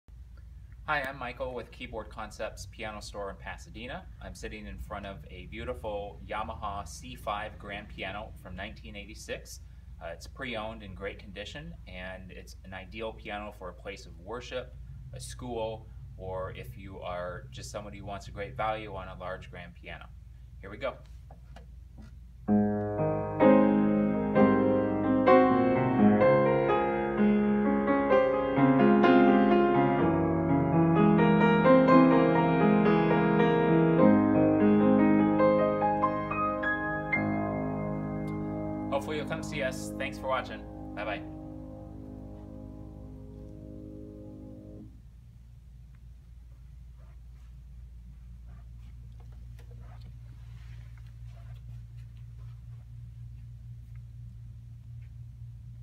Keyboard Classic of the Week: Yamaha C5 Grand - 1986
This pre-owned Yamaha 6'7" Grand Pianos is in great condition. We get the cream of piano trades and this beauty with its crisp, clear sound, won't be available for long.